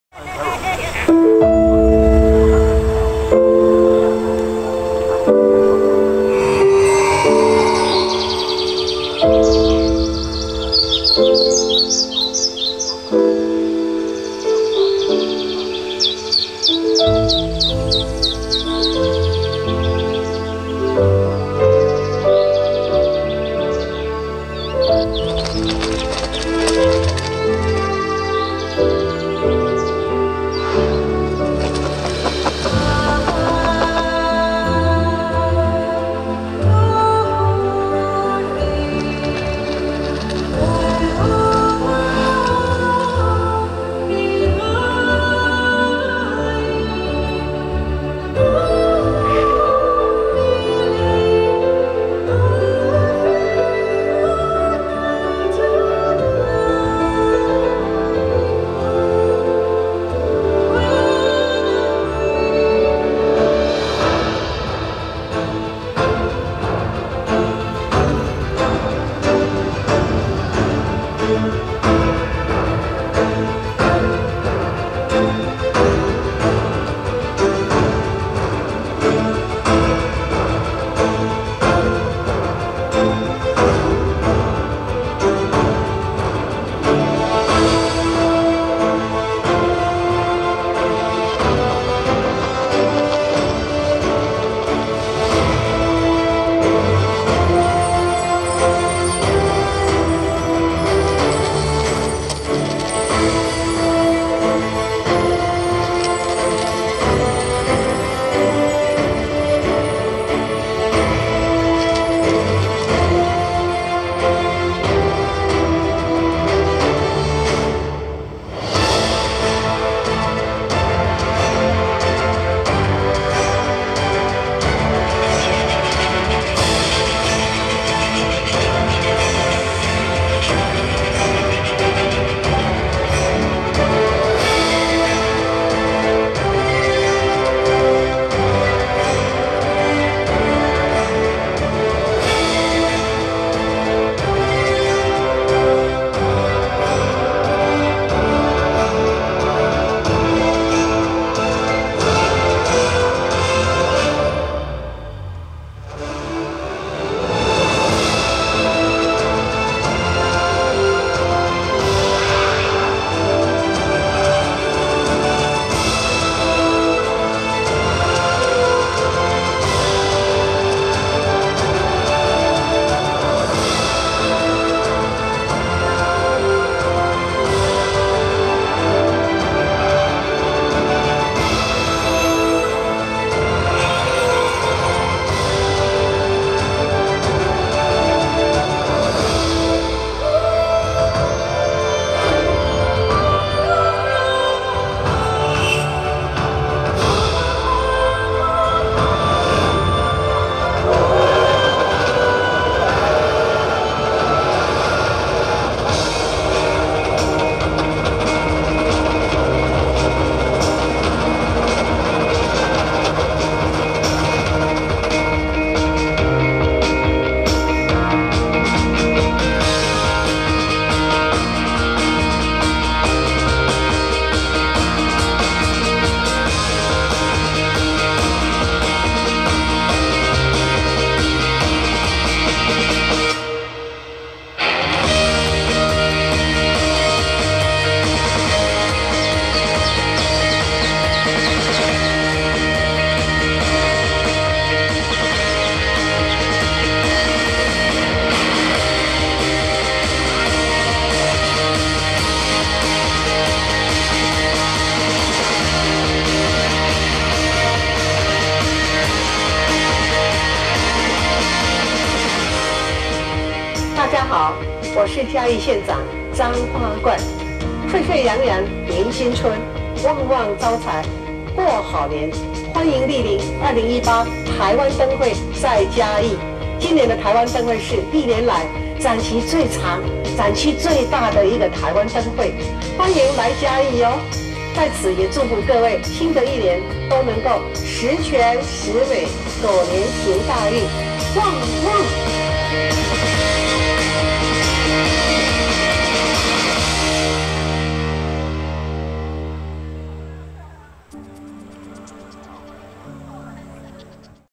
2018台灣燈會在嘉義-故宮南院 至美橋 水影舞集 遠景
2018台灣燈會在嘉義-故宮南院 至美橋 水影舞集  光雕秀

攝影器材:Sony HDR-PJ820
收音器材：RODE Video Mic Me

拍攝地點：嘉義國立故宮博物院南部院區